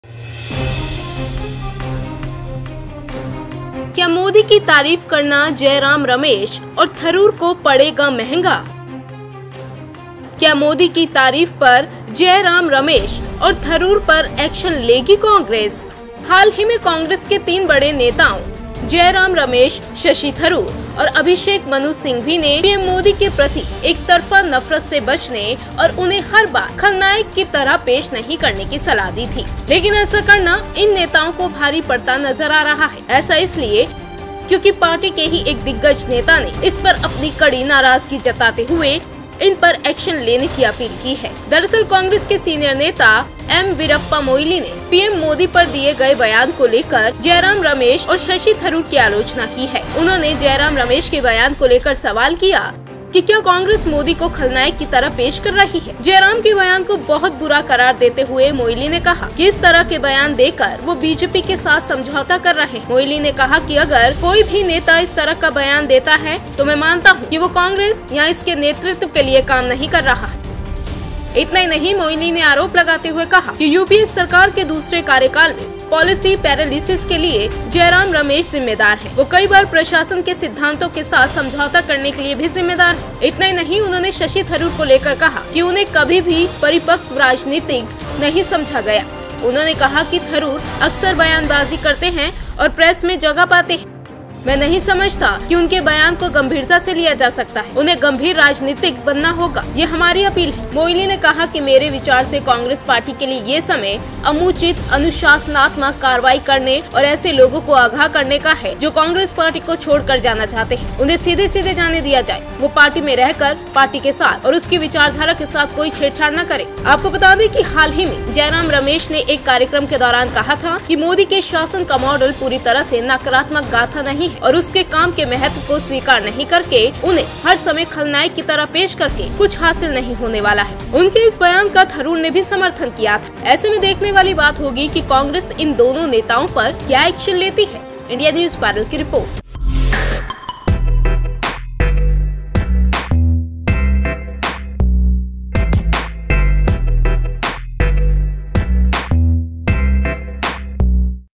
क्या मोदी की तारीफ करना जयराम रमेश और थरूर को पड़ेगा महंगा ? INDIA NEWS VIRAL
क्या मोदी की तारीफ करना जयराम रमेश और थरूर को पड़ेगा महंगा ? क्या मोदी की तारीफ पर जयराम रमेश और थरूर पर एक्शन लेगी कांग्रेस ? जानिए इस रिपोर्ट में